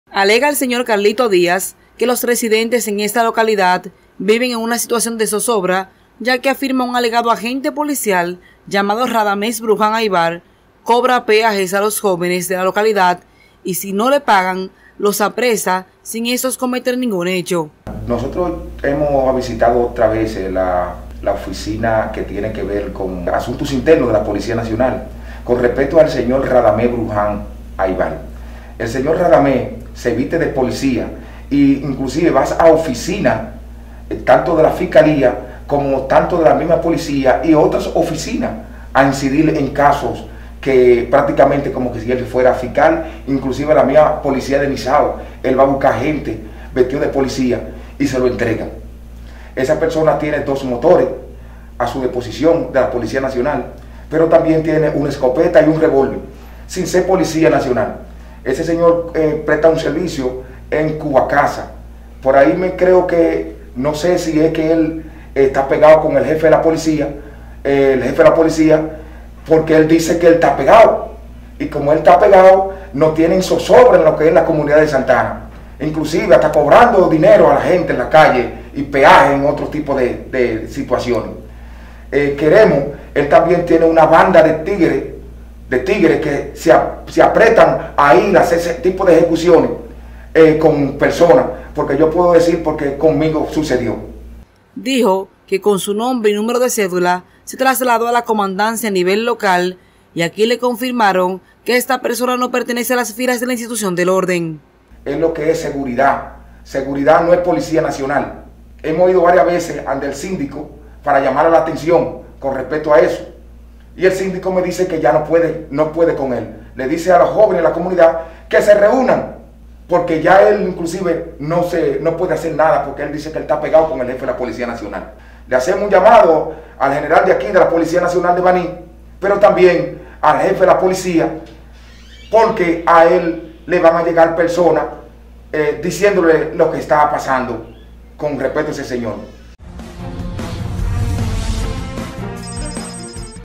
Denuncian hombre se hace pasar por policía cometiendo abusos contra residentes de Santana
Un señor residente en el distrito municipal de Santana denunció las supuestas arbitrariedades que comete una persona, la cual alega se hace pasar por miembro de la policía Nacional.